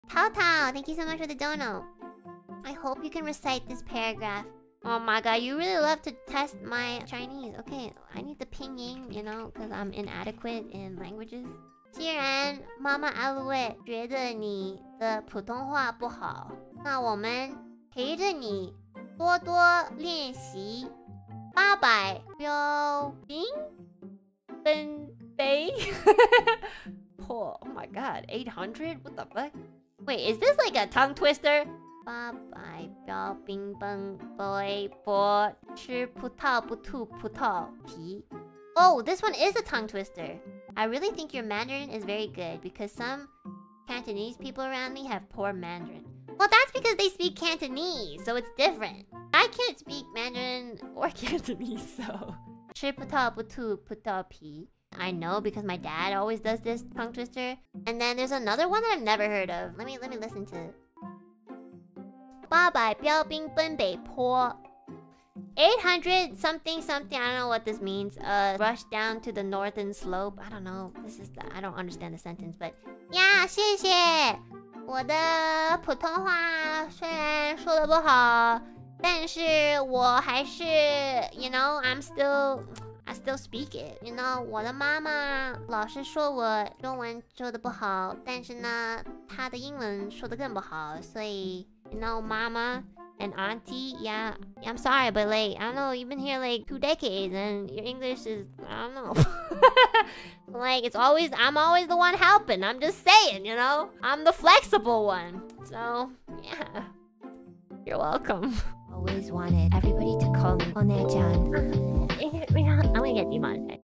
Enna's cute attempts at Chinese tongue twister reminds her of her beef with...
Those attempts are perfect I would say

ALSO CONTEXT: Enna immediately admitted defeat afterward, saying that both her mom and aunty speak multiple languages like Chinese, Cantonese, and Japanese

Hope you enjoyed the clip!